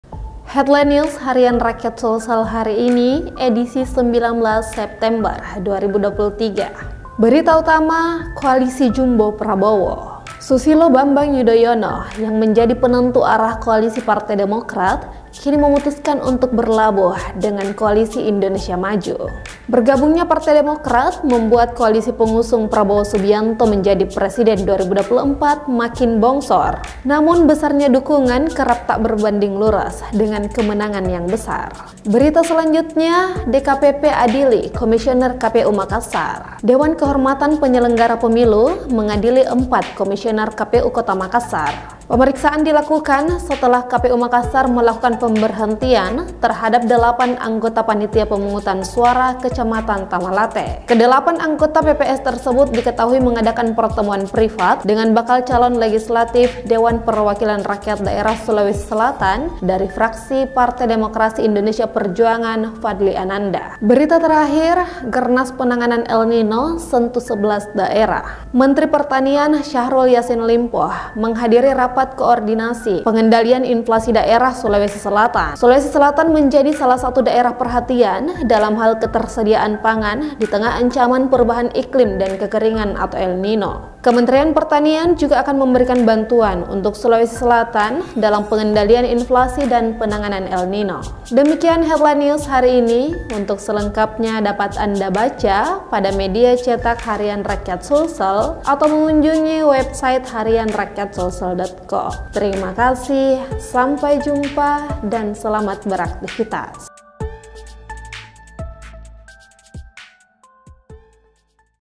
Koalisi Jumbo Prabowo | Headline News
Headline News Rakyat Sulsel 19 September 2023
1. Koalisi Jumbo Prabowo
2. DKPP Adili Komisioner KPU Makassar
3. Gernas Penanganan El Nino Sentuh Sebelas Daerah